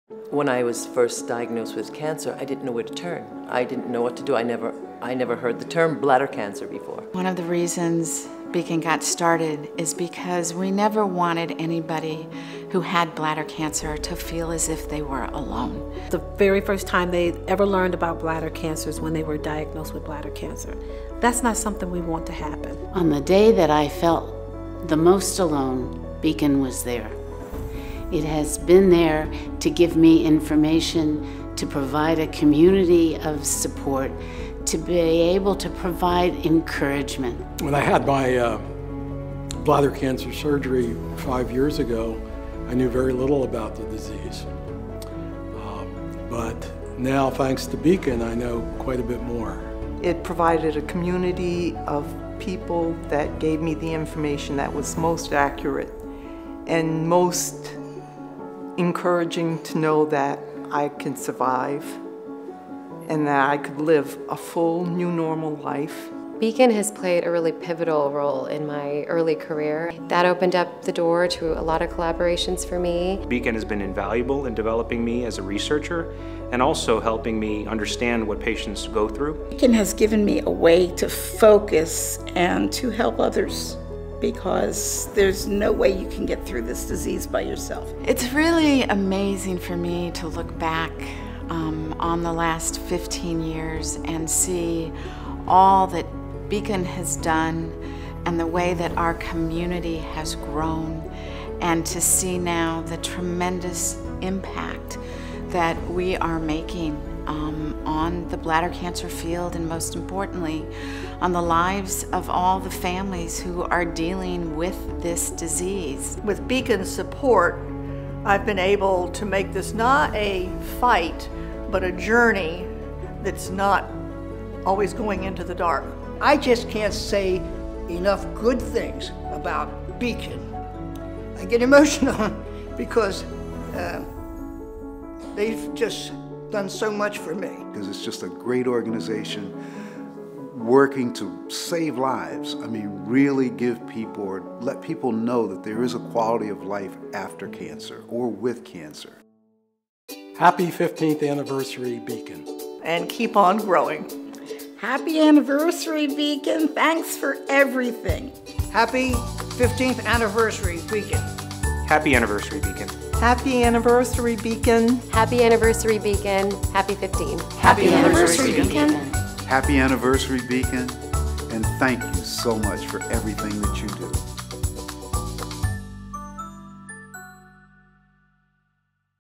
Bladder Cancer Advocacy Network Turns 15
BCAN patients, caregivers and others help salute the organization on its 15th anniversary.